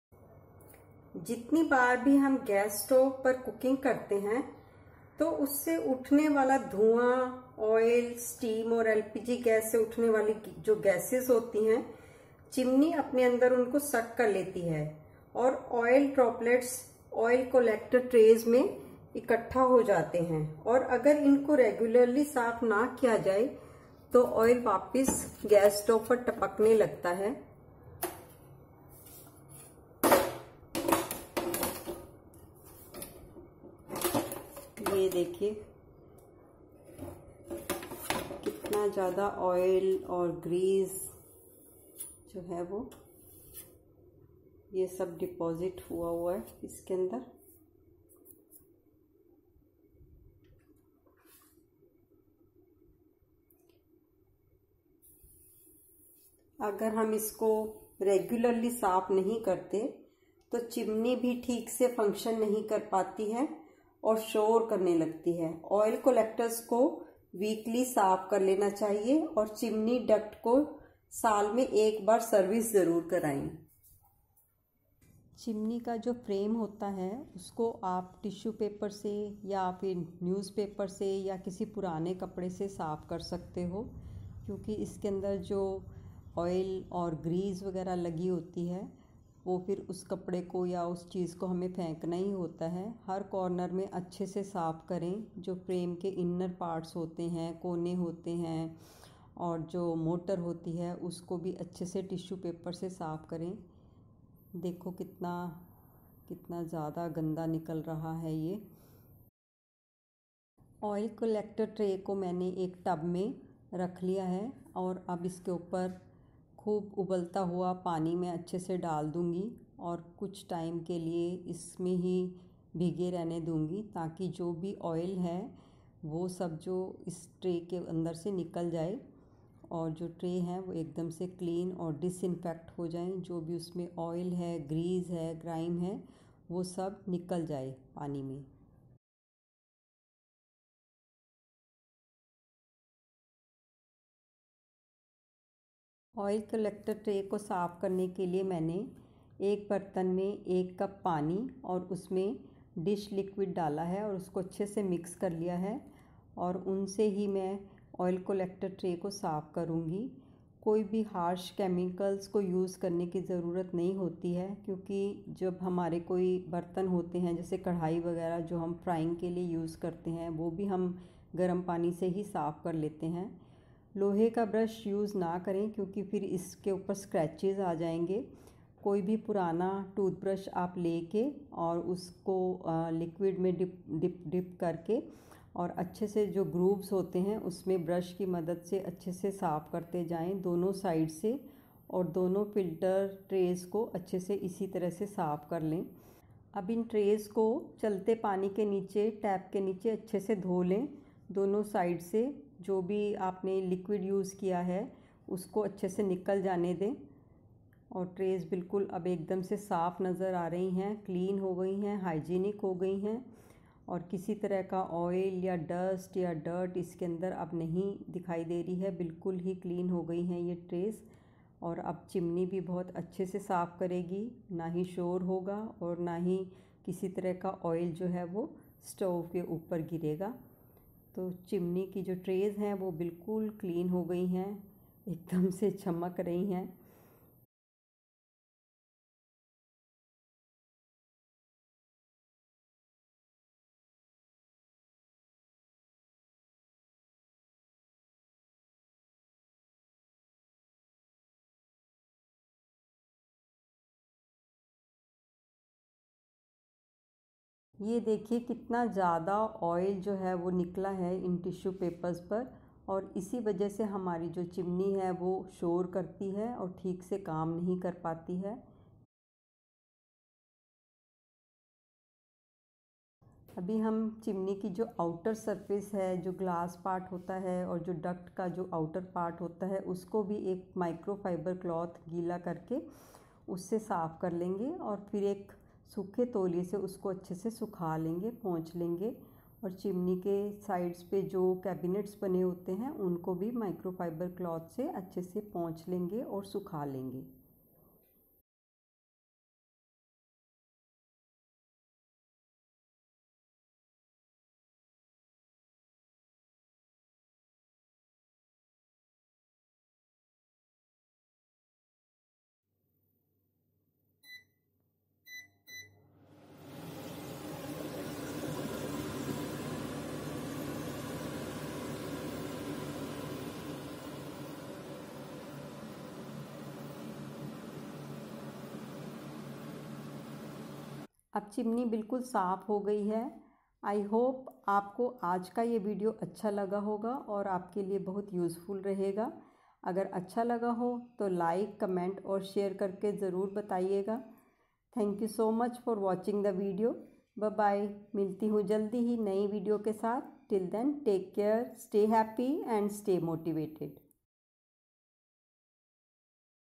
How to do CHIMNEY SERVICE all by yourself | Kaff India
Hey my friends! In this video I am talking about how you can clean your chimney all by yourself at home and trust me it's an easy process.